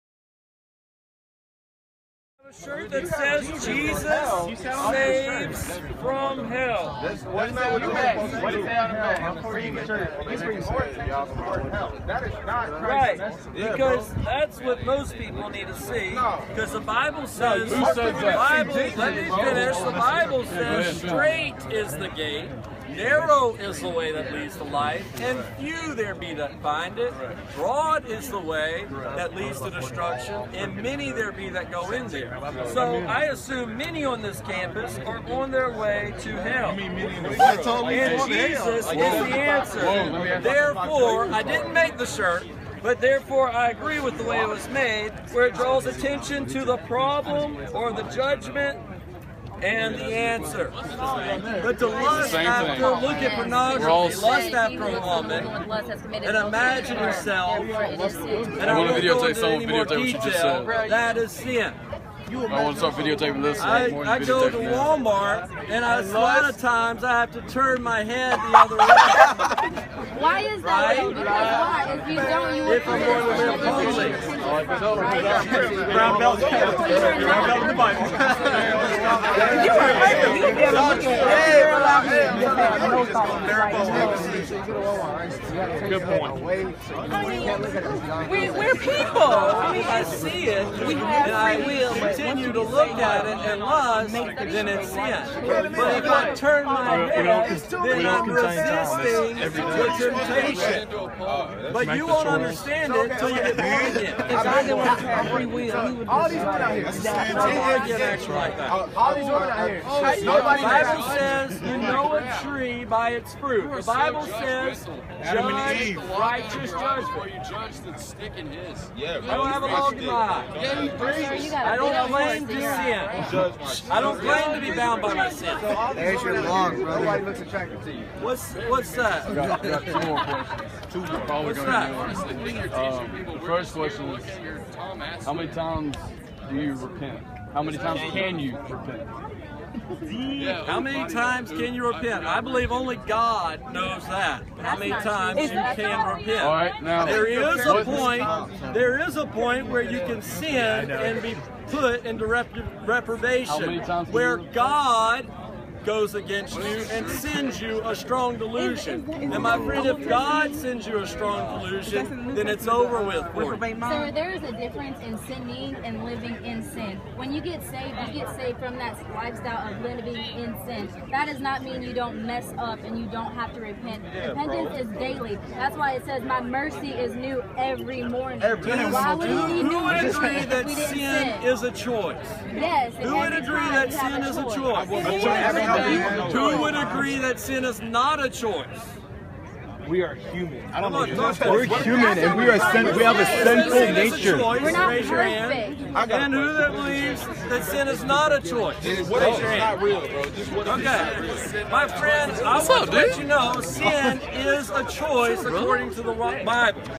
MSU Students Confront Campus Hate Preacher: "We're people!"
Mississippi State University students confronted a hate mongering street preacher on campus on April 8, 2015. While he warned that the majority of them would burn in hell, they sought to remind him of their shared imperfect humanity.